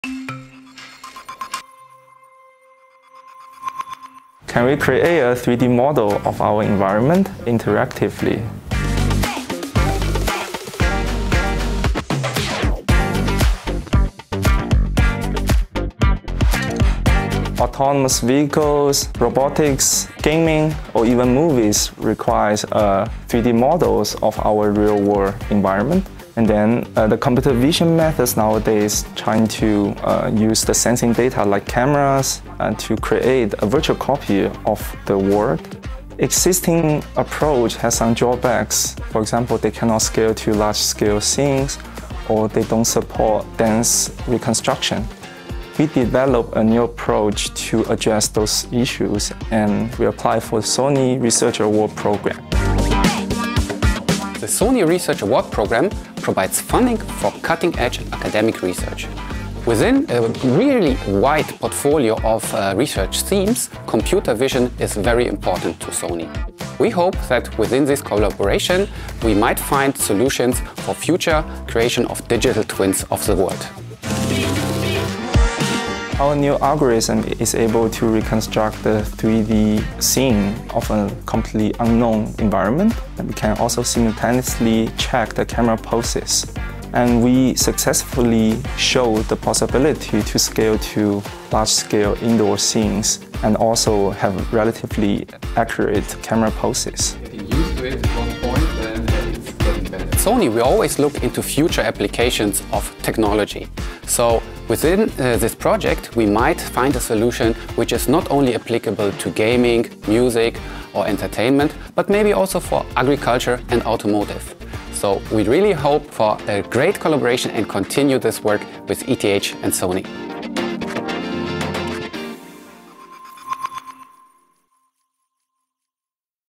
Virtual copy of the real world
Scanning the environment with a camera and interactively creating a virtual 3D model? ETH researchers receive funding from the Sony Research Award Program to advance their pioneering computer vision methods.